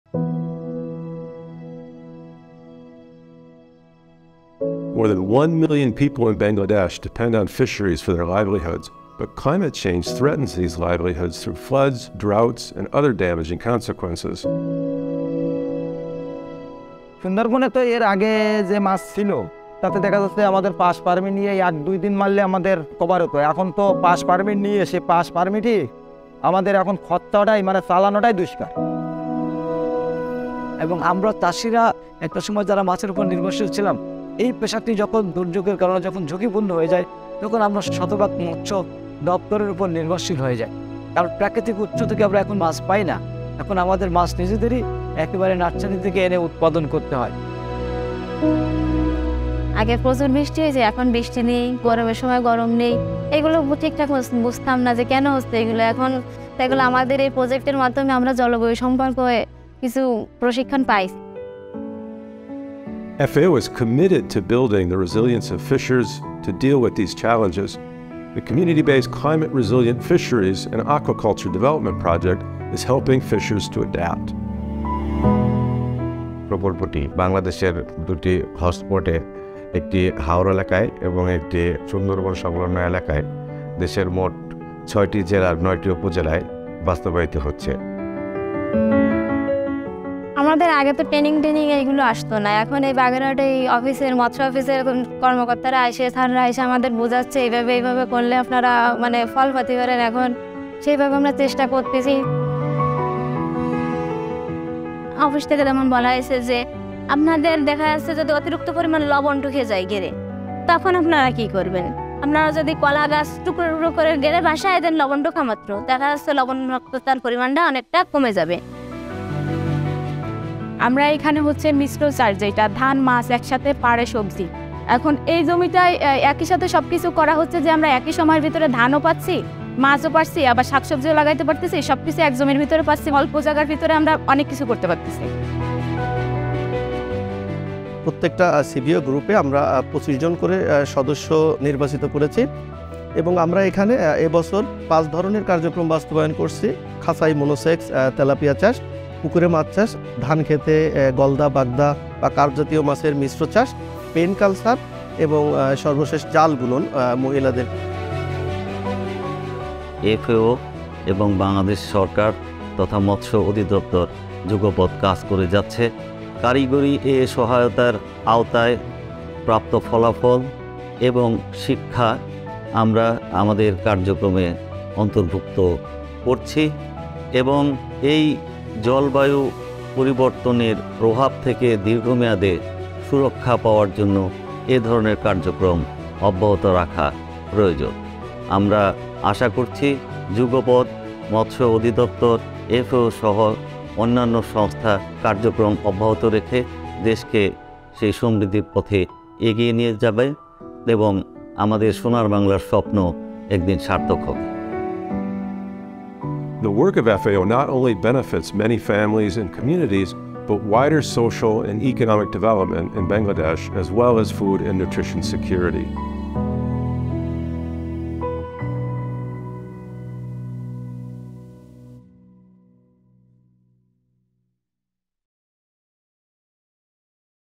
Bangladeshi fishers adapt to climate change
Climate change threatens the livelihoods of more than one million people in Bangladesh who depend on fisheries. FAO’s Community-based Climate Resilient Fisheries and Aquaculture Development project, funded by the Global Environment Facility, is helping fishers to adapt to challenges including droughts and floods.